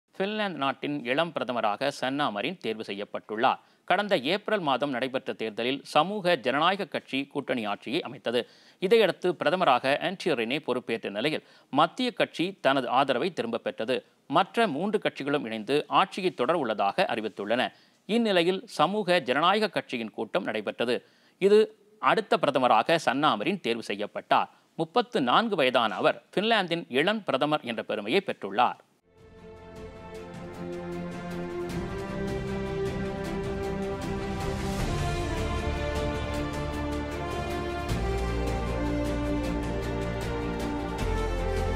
ஃபின்லாந்தின் பிரதமராக சன்னா மரின் தேர்வு
ஃபின்லாந்து நாட்டில் 34 வயதான சன்னா மரின் புதிய பிரதமராக தேர்வு.'